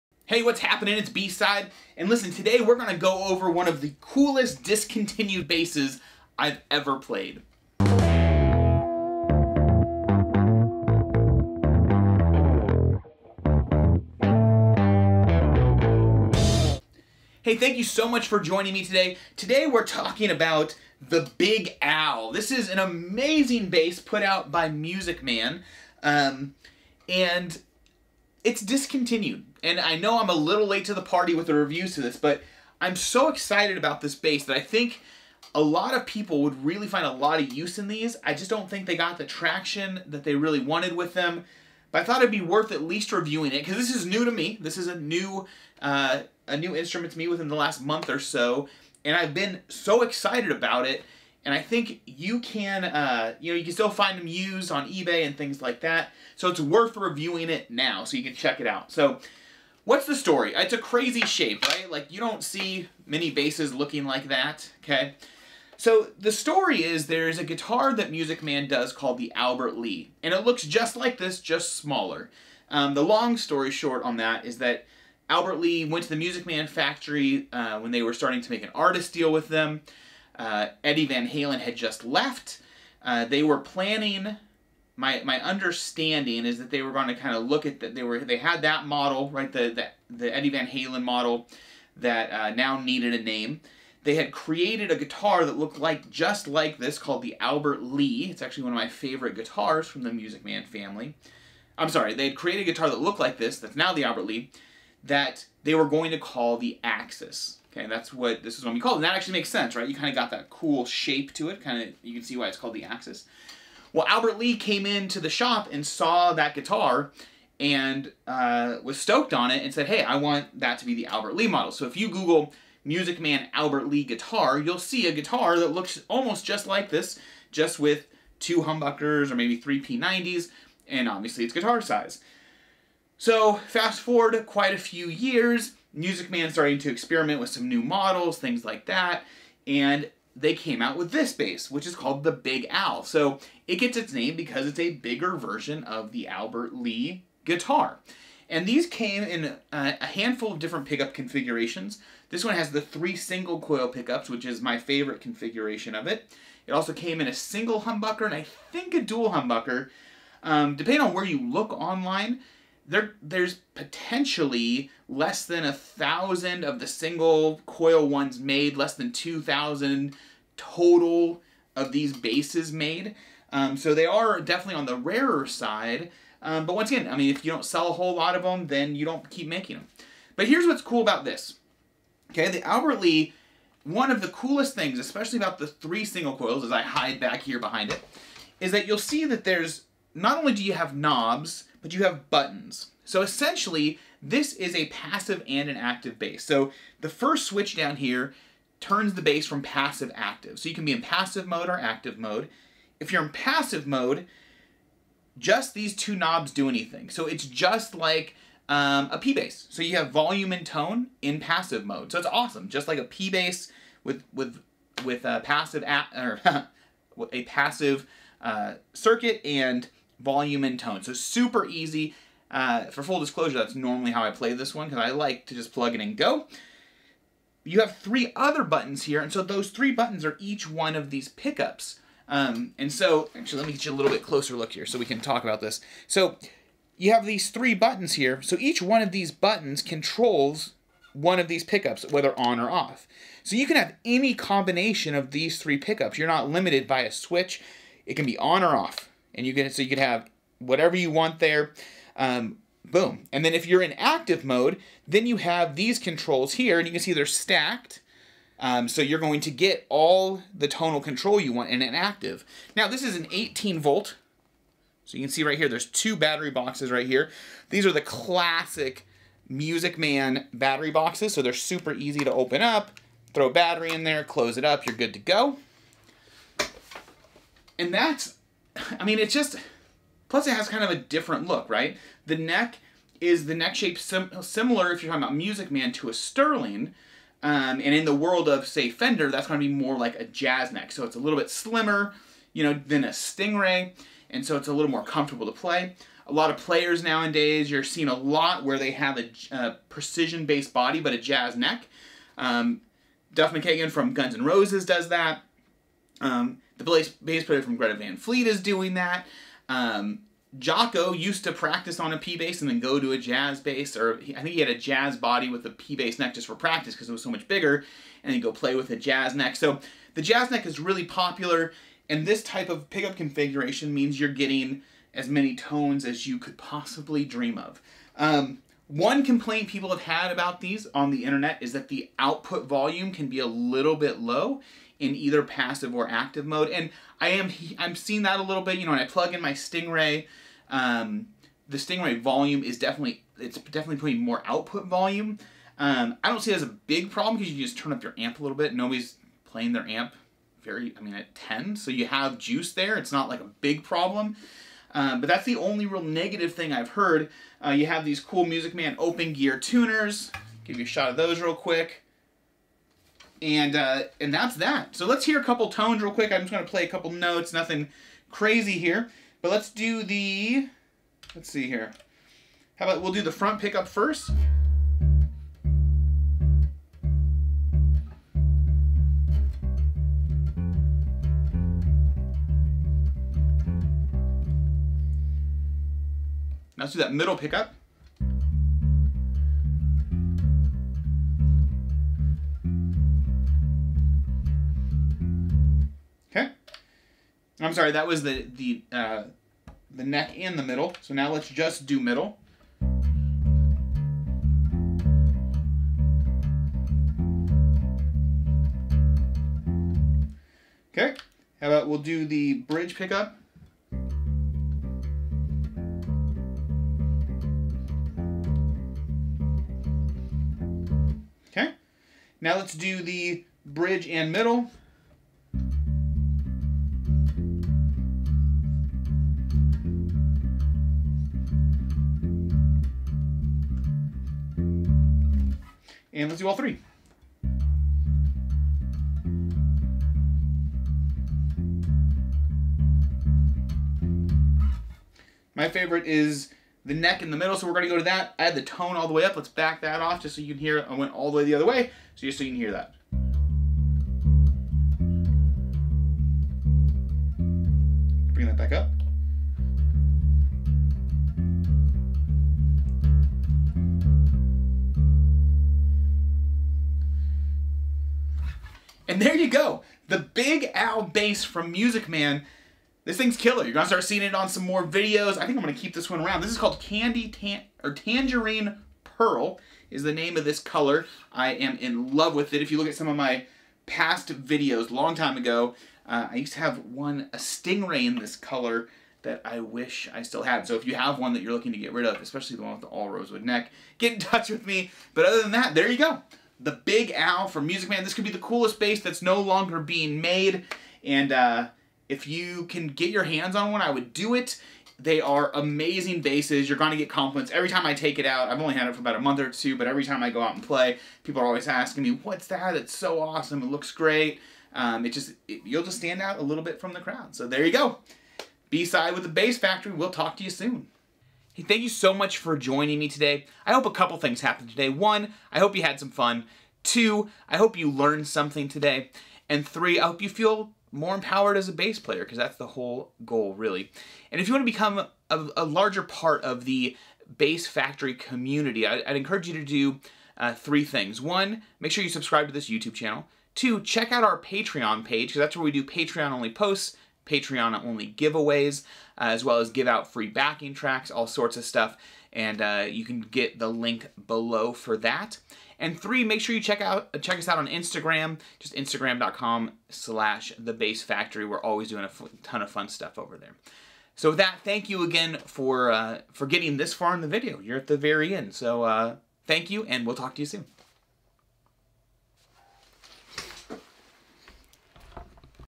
Music Man Big Al Bass Guitar Review | The Coolest Discontinued Bass on the Planet
You can't deny how cool the Music Man Big Al Bass is. Unfortunately, they did not sell all that well so they are discontinued. But if you find one, buy it! Its unique switching system allows for an array of tones and it looks amazing!